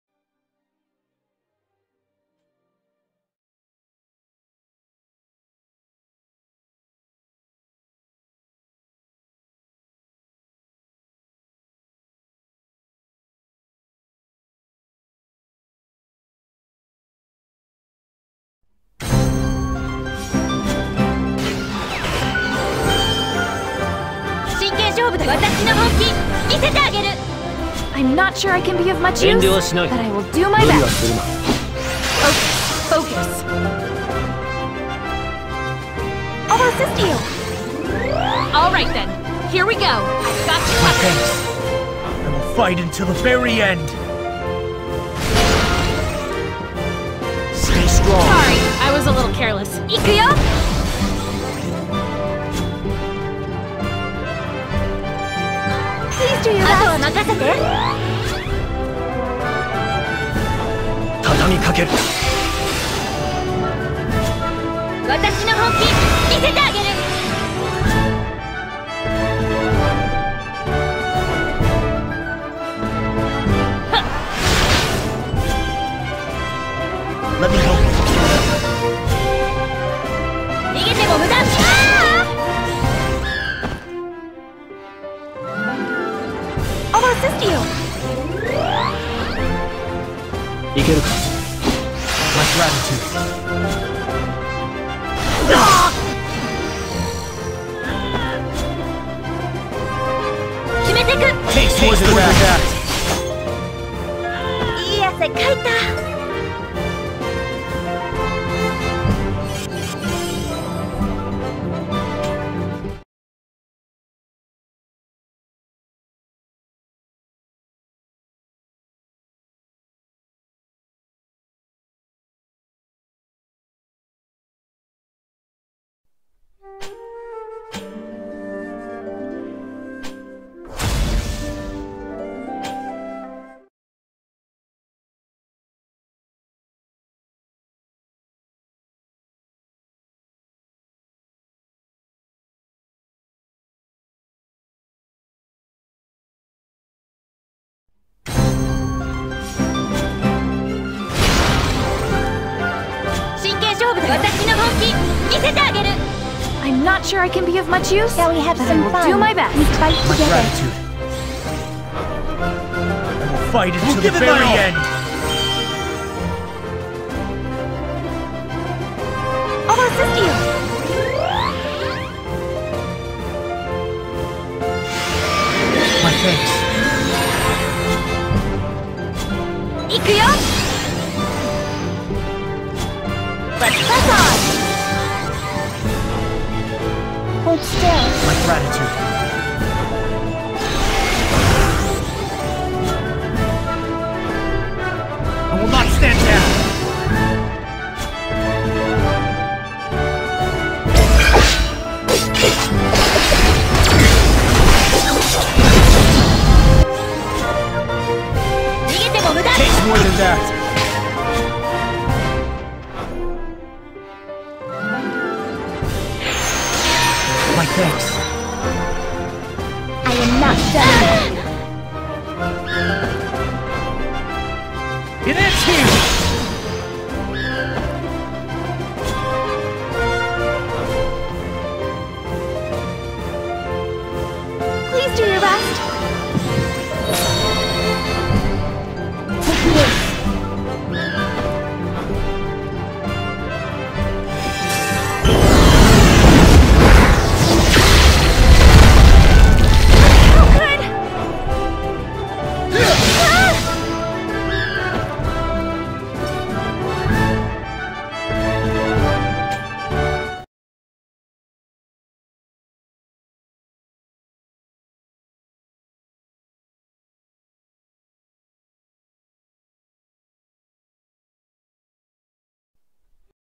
Guild Battle 43 | 2S vs 光復香港#時代革命
Battle 1: Cloud, Frederika, King Mont
Battle 2: Kilphe, King Mont, Yuna